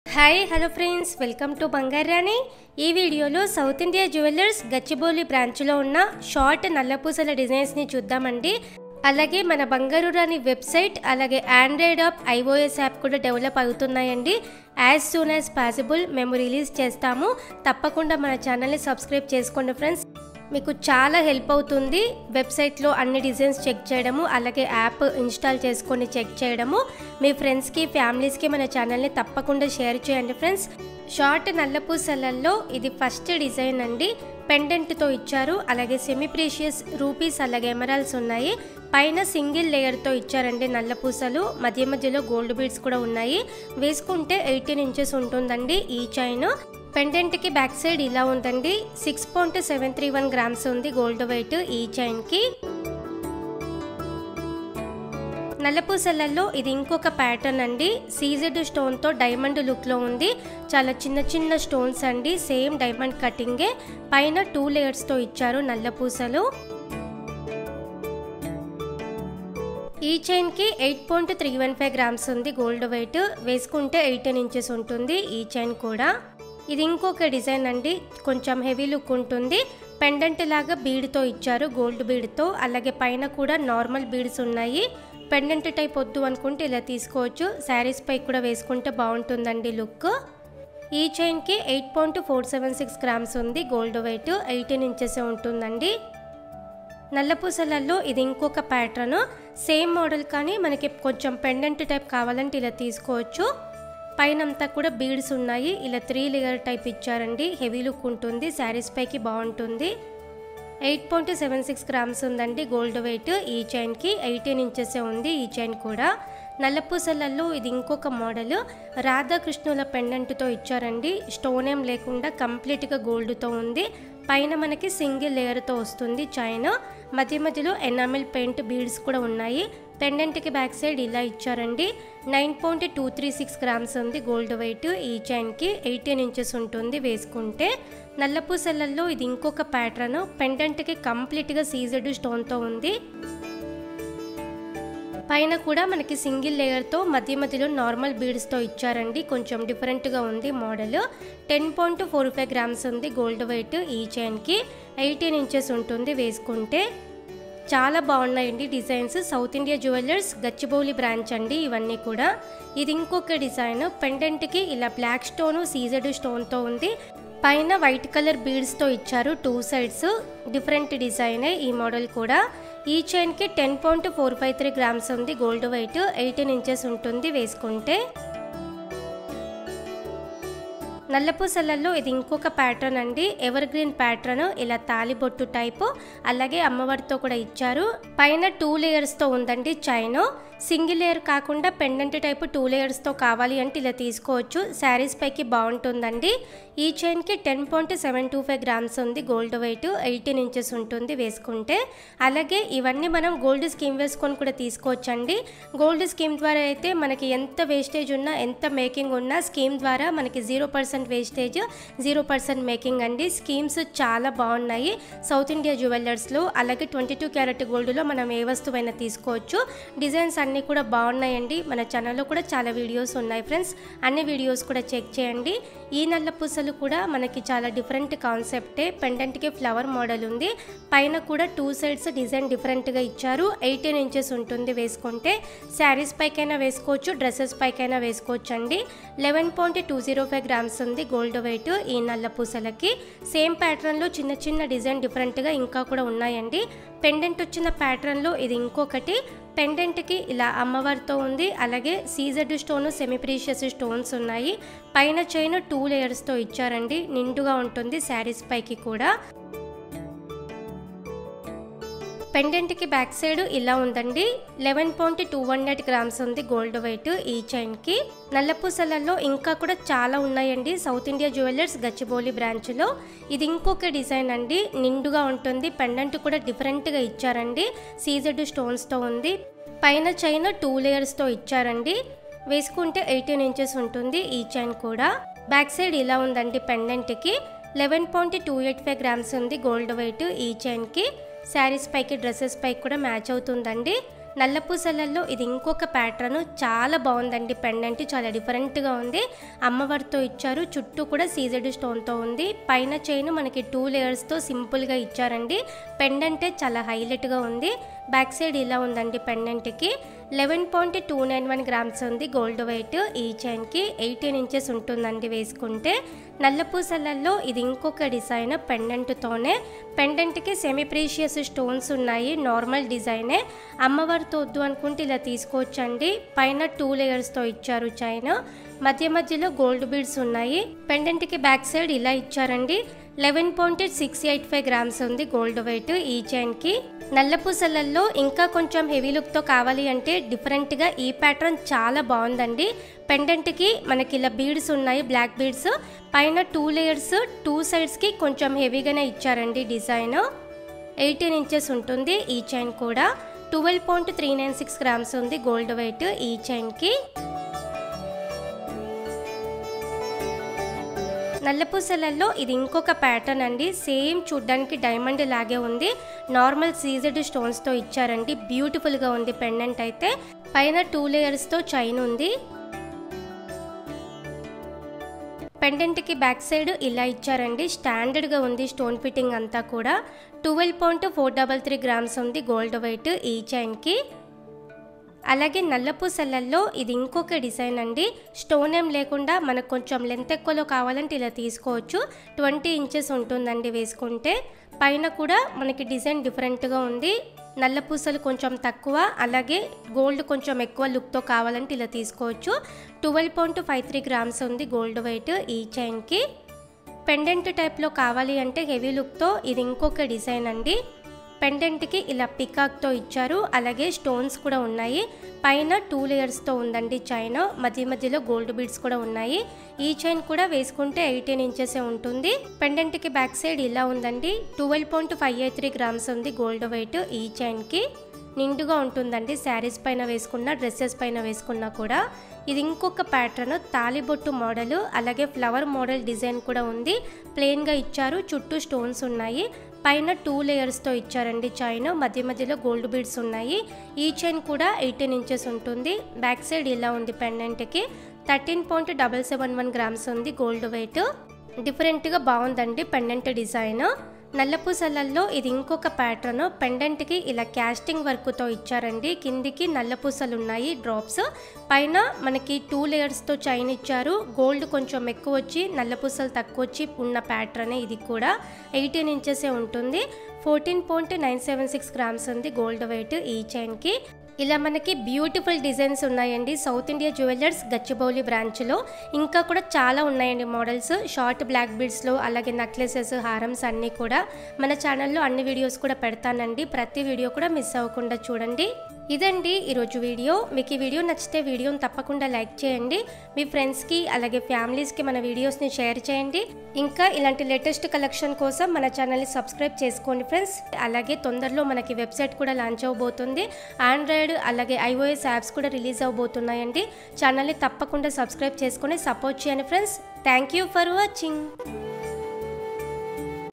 Nallapusalu new collection with detailed price | Blackbeads designs | gold nallapusalu collection
Hi Friends,

This video is from
Southindia Jewellers
Gachibowli Branch
Mr. Ramana
Branch Manager

Please contact him for more queries.